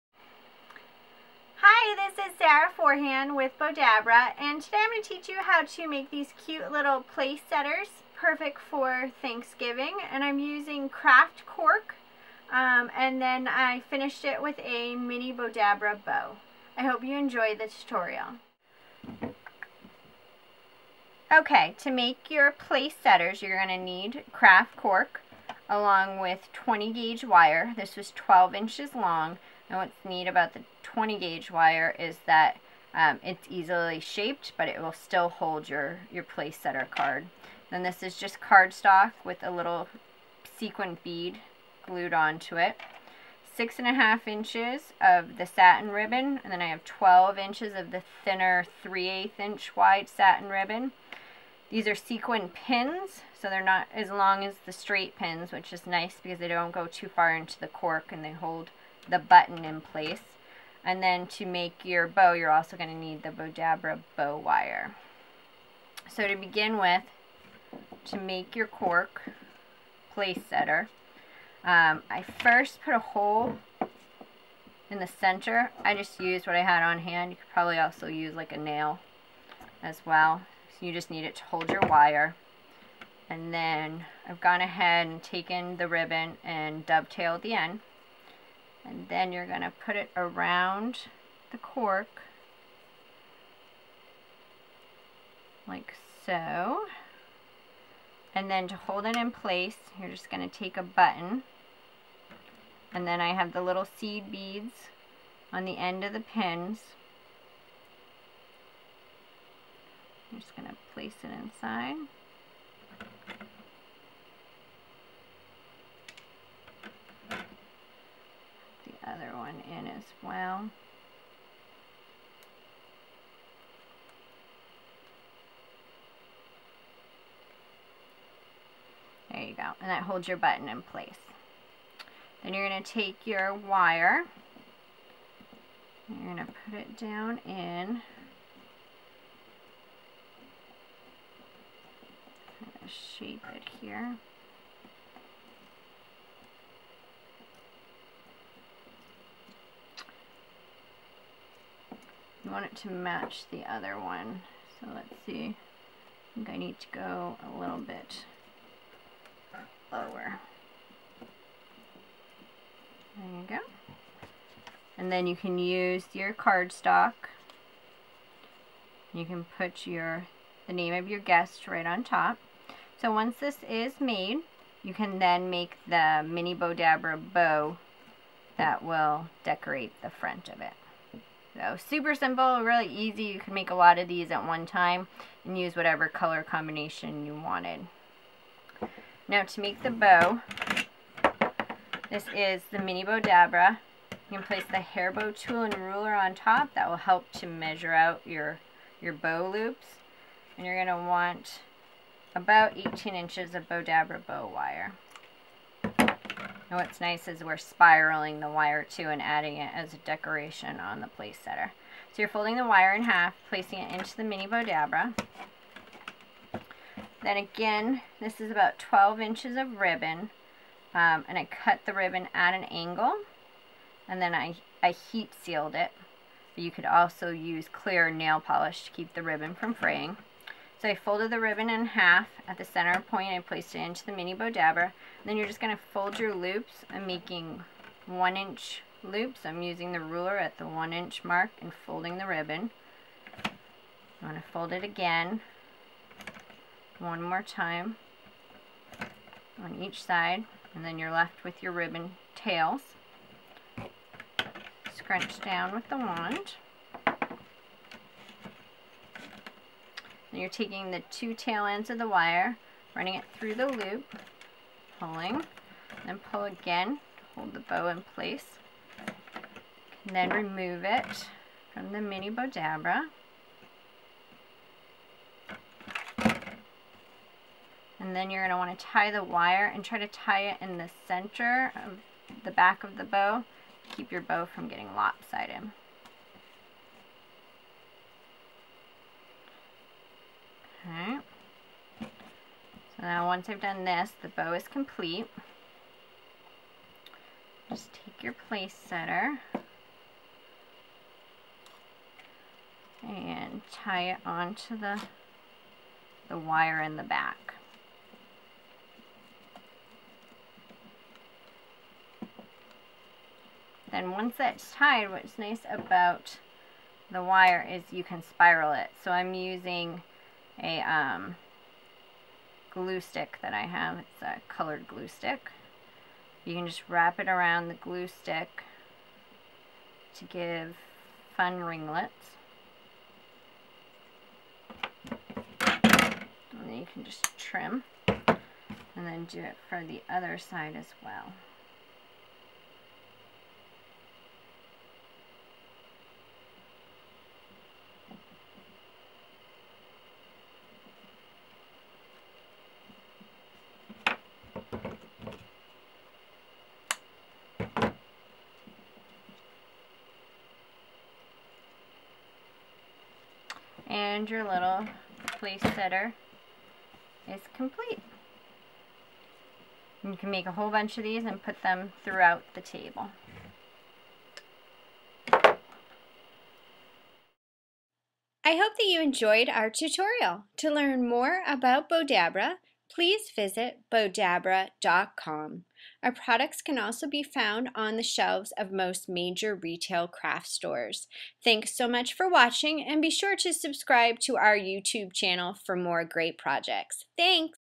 Make Your Own Table Place Setters with Mini Bowdabra Bow
Design your own place setters and finish with a Mini Bowdabra Bow.  Perfect for your holiday table this Thanksgiving.  To learn more about Bowdabra Bow Making products please